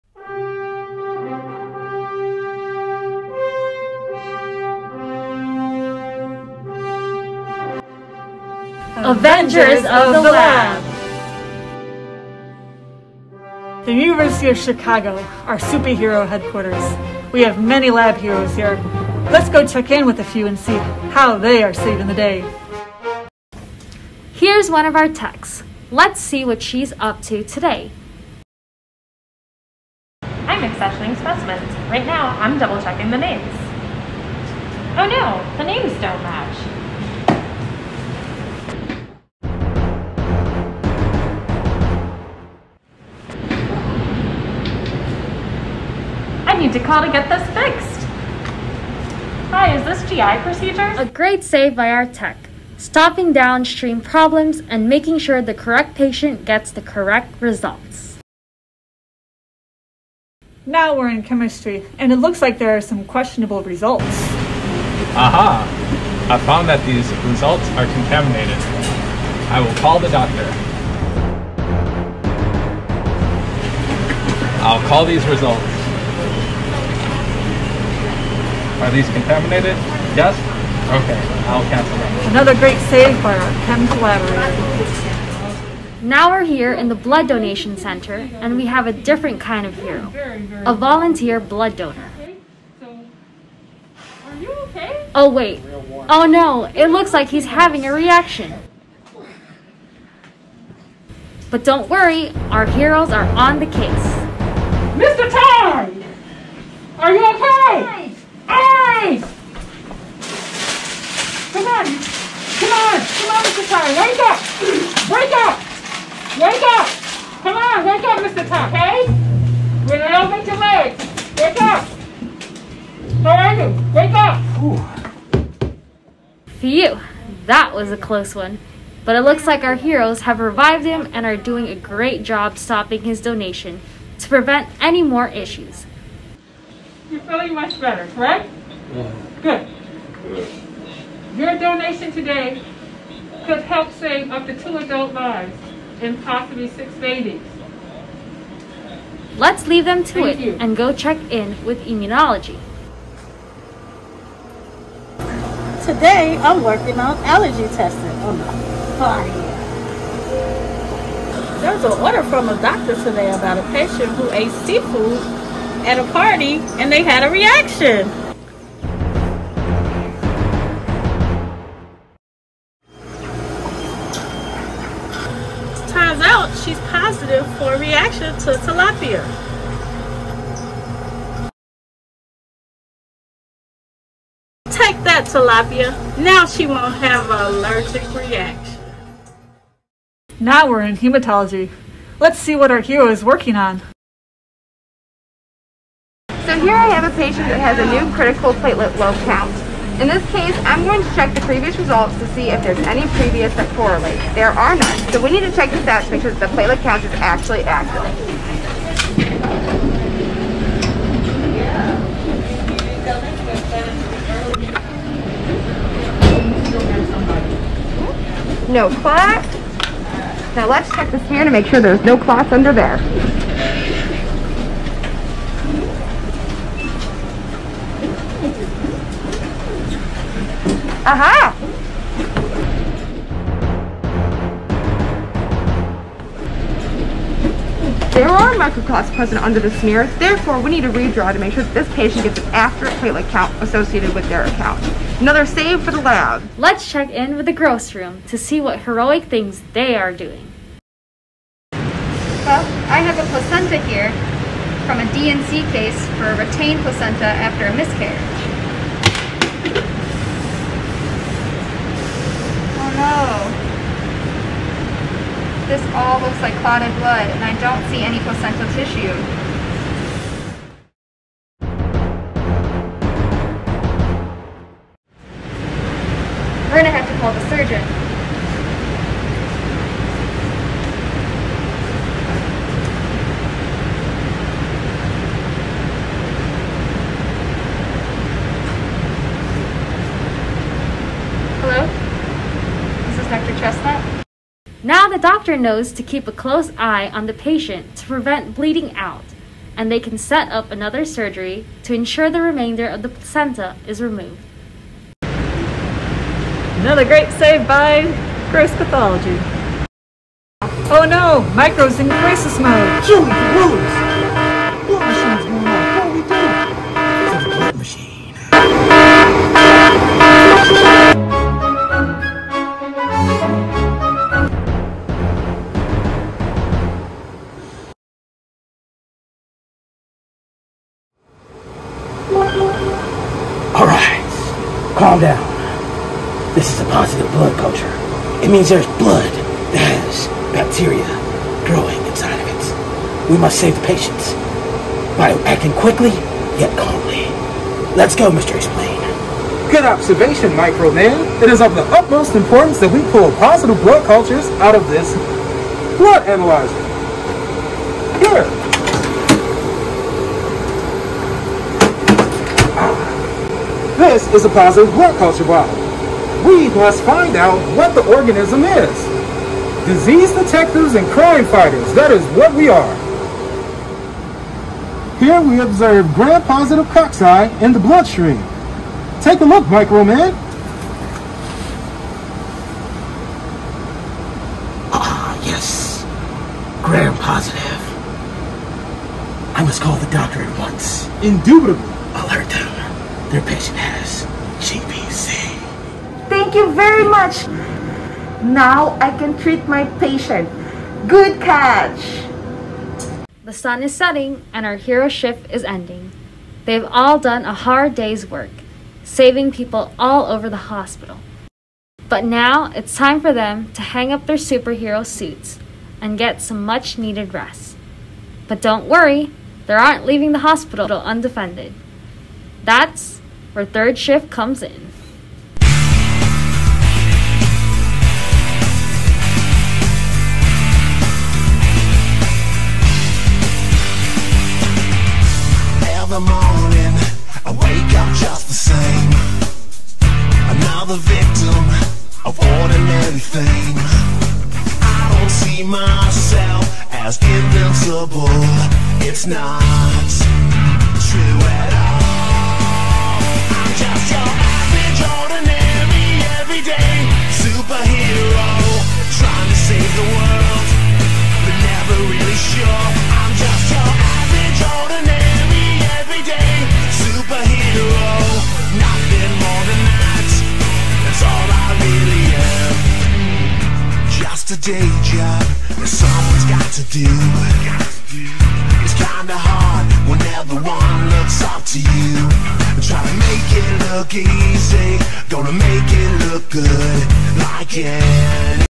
Everyday SuperHeroes - University of Chicago Medicine Laboratories- Lab Week Video
Submission for the 2021 Avengers of the Laboratory Photo and Video Contest